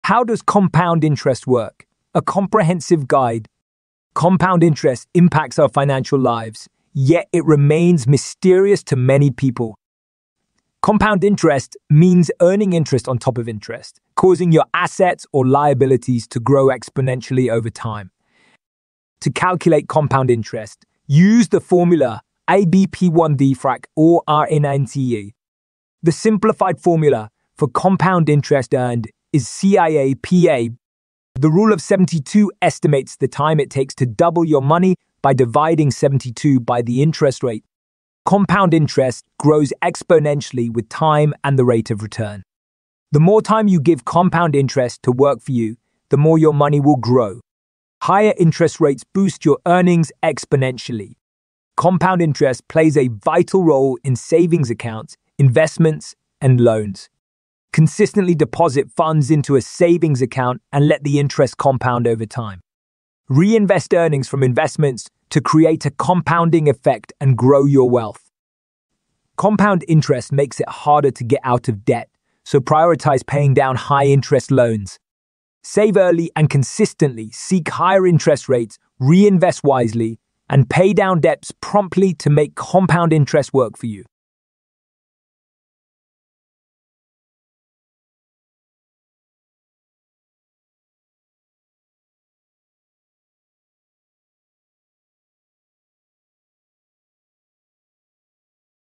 How Does Compound Interest Work: A Comprehensive Guide
Compound interest is a fundamental financial concept that impacts almost every aspect of our financial lives, yet it remains mysterious to many people. Understanding how compound interest works helps build a strong foundation for personal finance and supports long-term financial growth. From your everyday savings account to managing debt and investment planning, this comprehensive guide is designed to help you grasp the mechanics and benefits of compound interest.